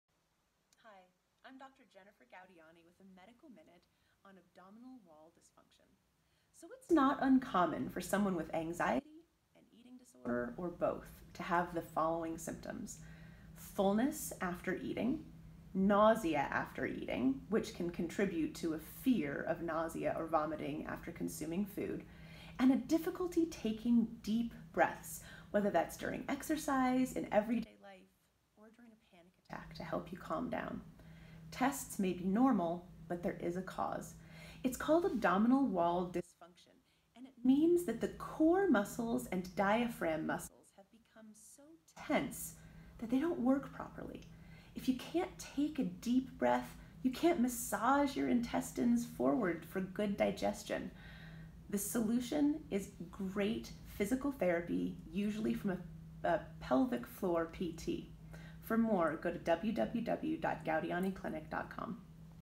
Medical Minute: Abdominal Wall Dysfunction
Dr. Jennifer Gaudiani explains abdominal wall dysfunction and how people with eating disorders can be affected by this issue.